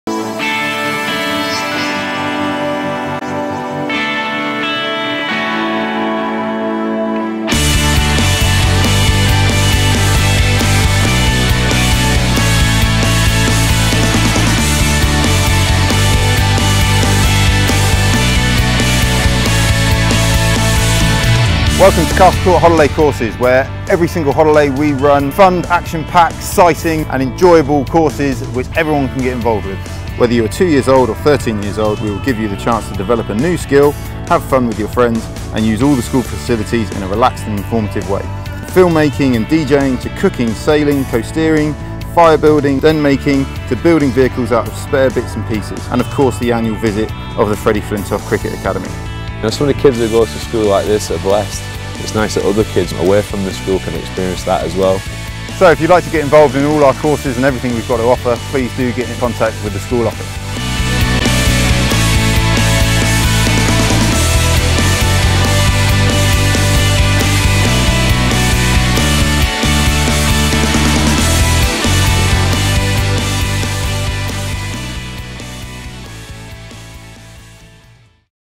Holiday Courses Dorset - Castle Court School
Children's Holiday Courses are available at Castle Court School, full of fun packed activities to keep your children  entertained during the school holidays.
Dorset private school, Castle Court offer well organised and exciting activities during school holidays.  These activities are not limited to pupils of the school and are open to all children, from aged 2 to 13,  in the local area.